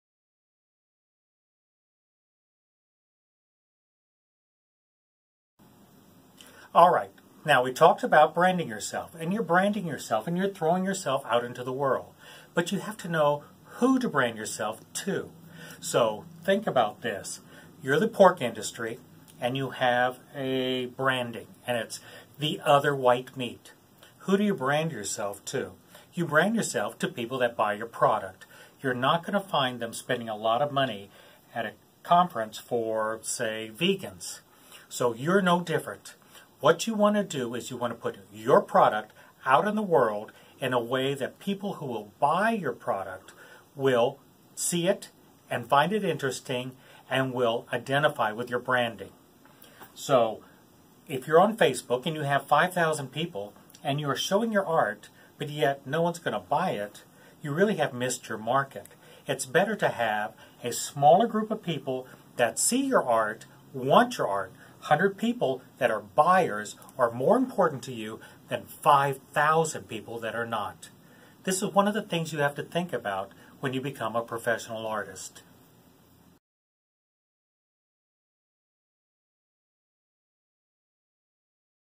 Episode 4: "Selling to the correct buying market"
Here it is the 4th video in my artist series:
In this 3/15/17 video: Professional artist, Michael Tolleson, Autistic Savant, addresses "Promoting your art to the correct market"